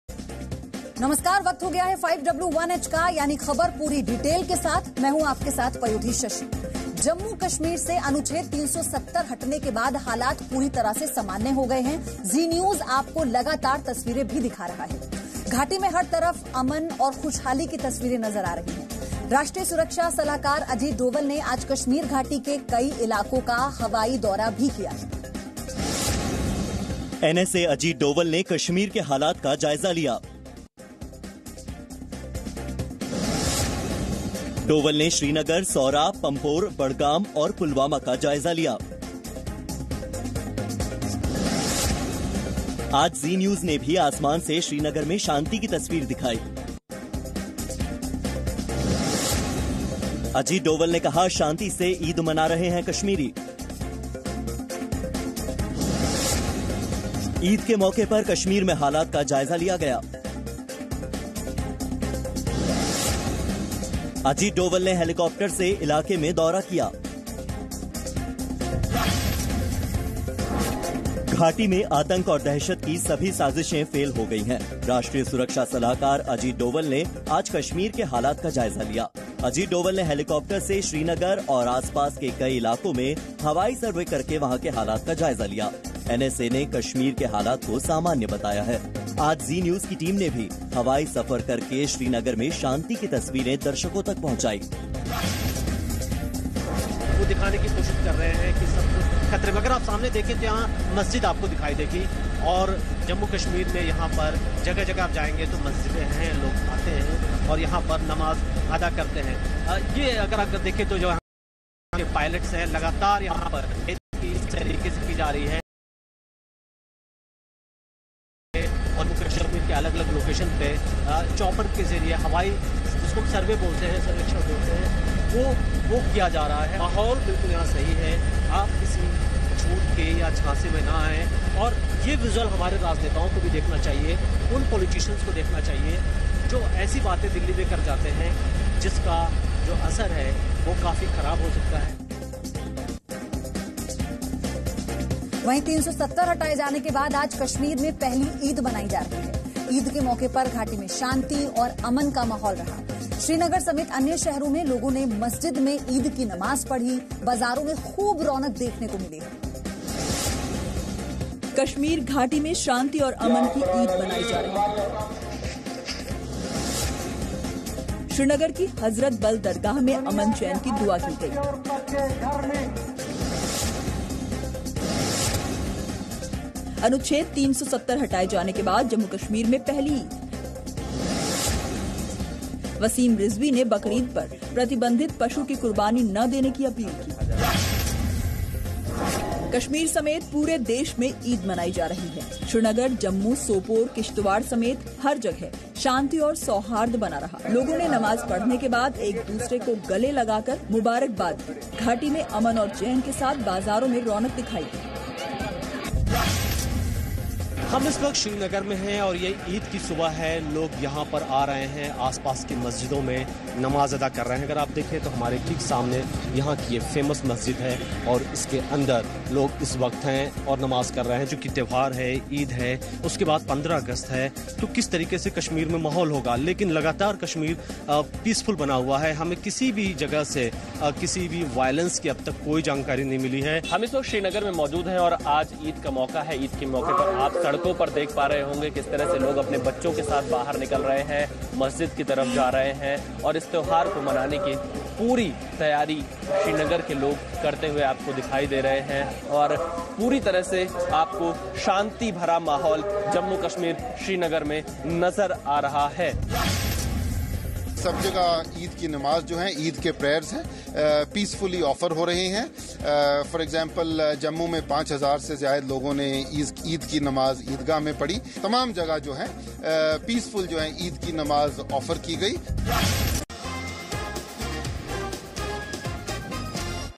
NSA Ajit Doval undertakes aerial survey of Kashmir Valley on Eid-al-Adha
National Security Advisor Ajit Doval on Monday undertook an aerial survey of the Kashmir Valley to take stock of the situation on the occasion of Eid-al-Adha. Watch this video to know more.